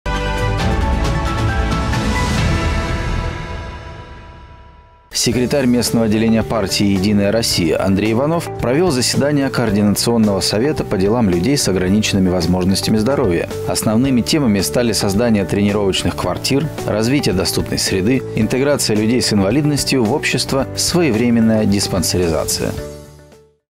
Координационный совет
Секретарь местного отделения партии «Единая Россия» Андрей Иванов провел заседание координационного совета по делам людей с ограниченными возможностями здоровья. Основными темами стали создание тренировочных квартир, развитие доступной среды, интеграция людей с инвалидностью в общество, своевременная диспансеризация.